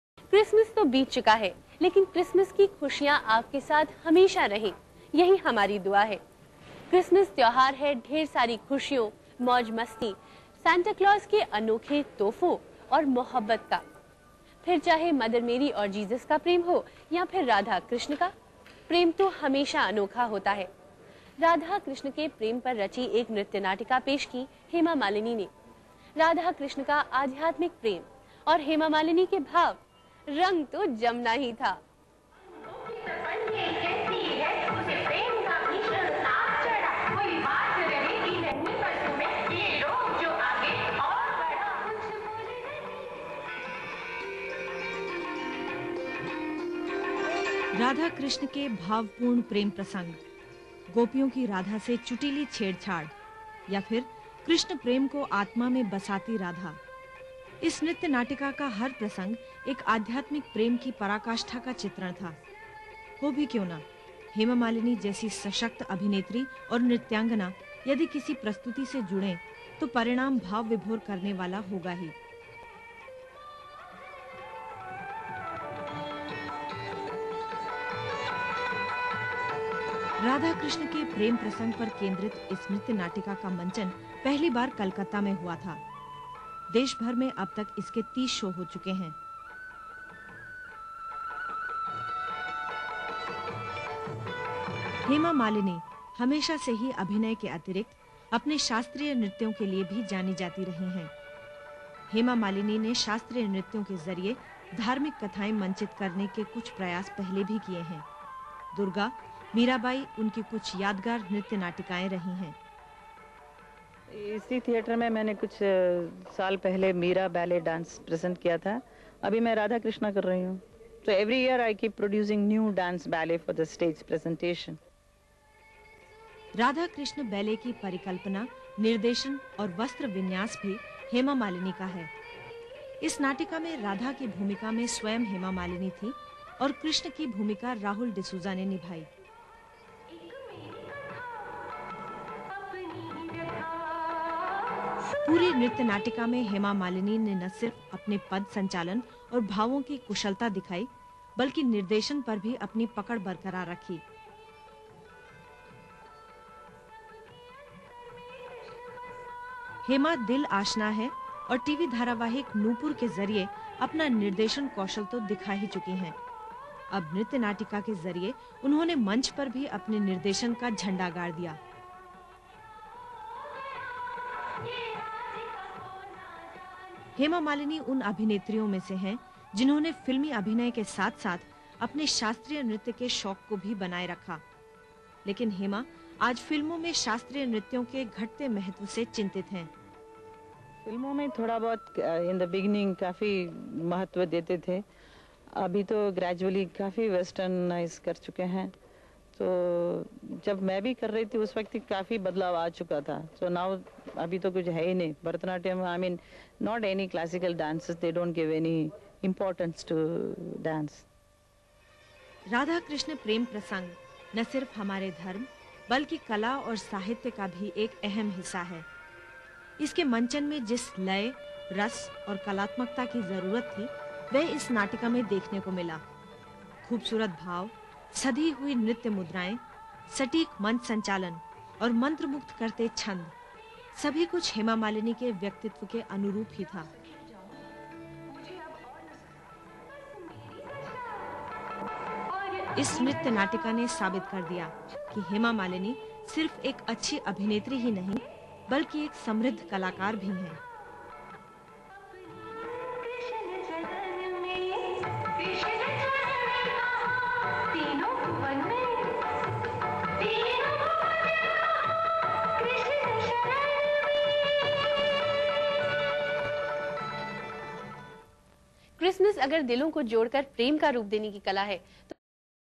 Dances by Hema Malini
Some shots of dance dramas choreographed by Hema Malini, doyen of film & stage & classical dancer.
From Z TV